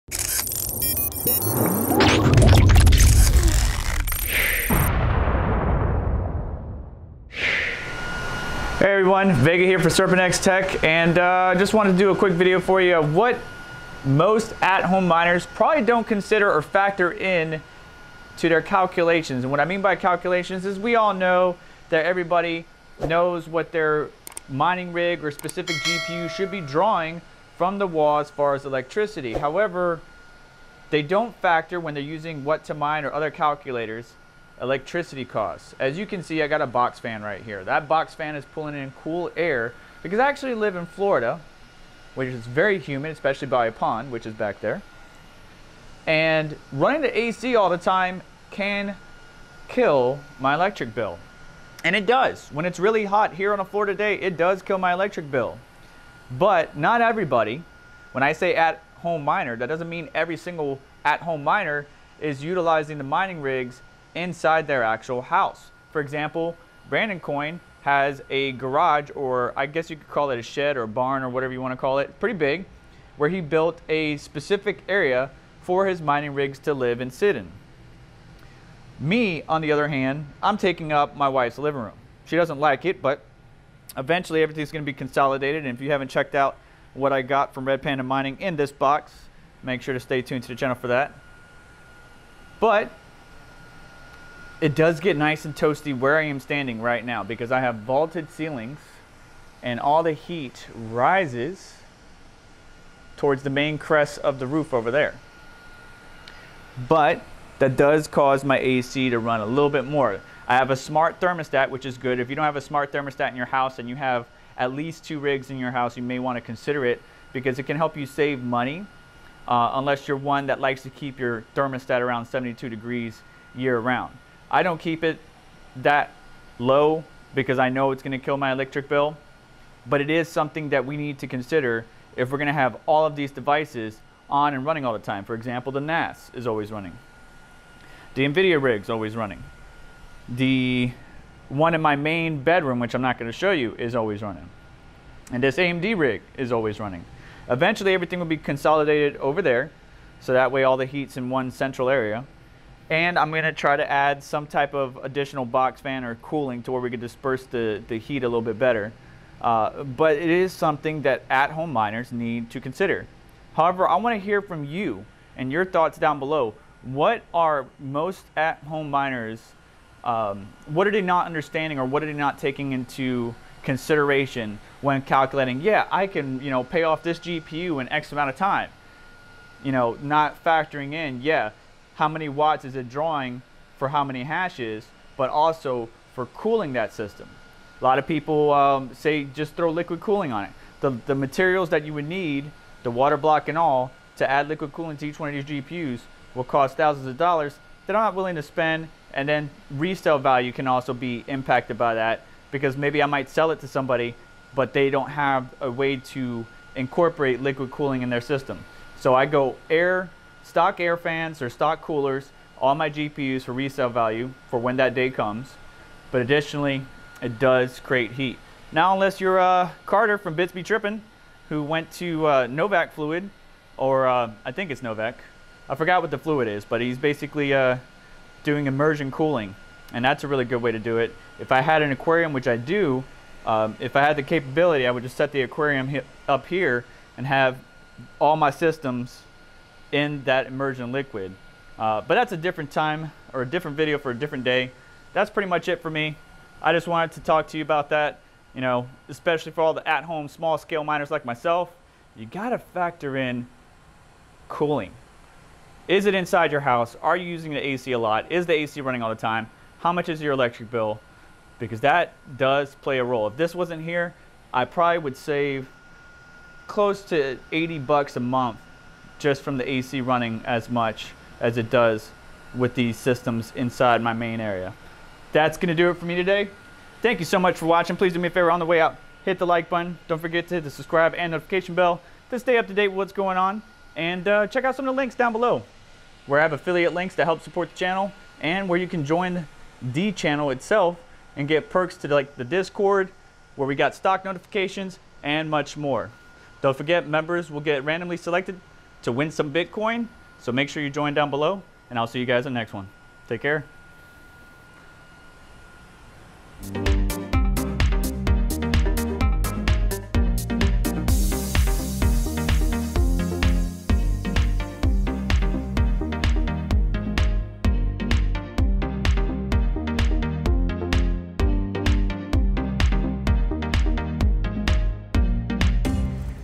What Most At-Home Miners don't Consider
I want to hear from you, and for you all to share with each other any insight, down in the comments below. We all know to check power draw, tune our GPU's, manage GPU thermals, and etc. However, what do we forget, or are we less concerned about that could factor into the overall cost for our setups?